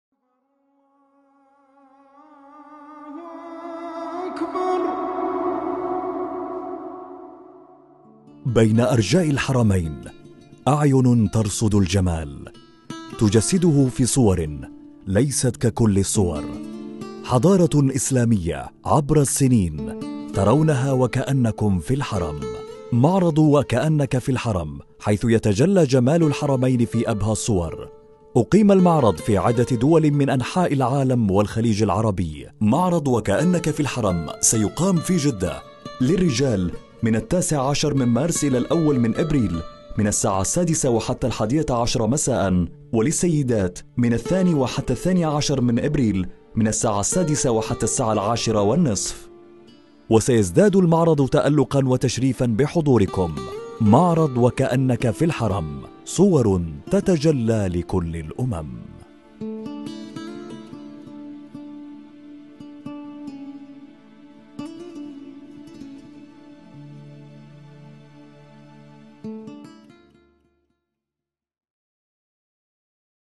معرض وكأنك في الحرم
معرض وكأنك في الحرم في مدينة جدة
للرجال: 19 مارس - 1 أبريل من الساعة 6-11 مساءً
للسيدات: 2 - 12 أبريل من الساعة 6 - 10:30 مساء
في مبنى ديار السلام - شارع الأمير سلطان خلف آيس لاند